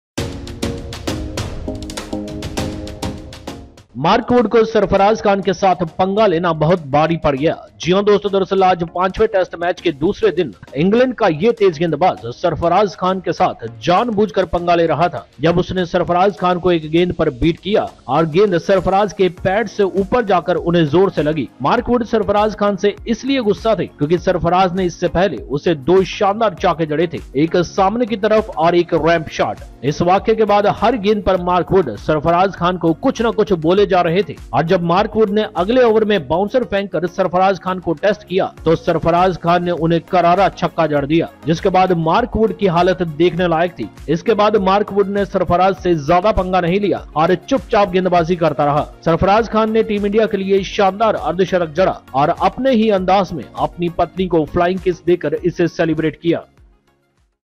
Mark Wood Sledging with Sarfaraz Khan after boundaries hit by Young Man Ind vs Eng 5th Test
Mark Wood found himself in a tough situation after engaging with Sarfaraz Khan in today's second day of the 5th Test match. Yes, friends, in fact, during the match, England's fast bowler Mark Wood was intentionally having a heated exchange with Sarfaraz Khan. When he beat Sarfaraz Khan with one delivery, and the ball hit his pads forcefully, Wood was visibly upset. The reason for Mark Wood's frustration was that before this incident, Sarfaraz had played two splendid boundaries against him—one straight down the ground and the other a ramp shot.

After this incident, on every delivery, Mark Wood was seen saying something to Sarfaraz Khan, trying to get under his skin. When Mark Wood bowled a bouncer to test Sarfaraz Khan in the next over, Sarfaraz responded by hitting a powerful six. This left Mark Wood in a difficult position. Following this, Mark Wood did not engage in further altercations with Sarfaraz and continued to focus on his bowling. Sarfaraz Khan played a fantastic innings for Team India, celebrating in his own style by giving a flying kiss to his wife after reaching a milestone.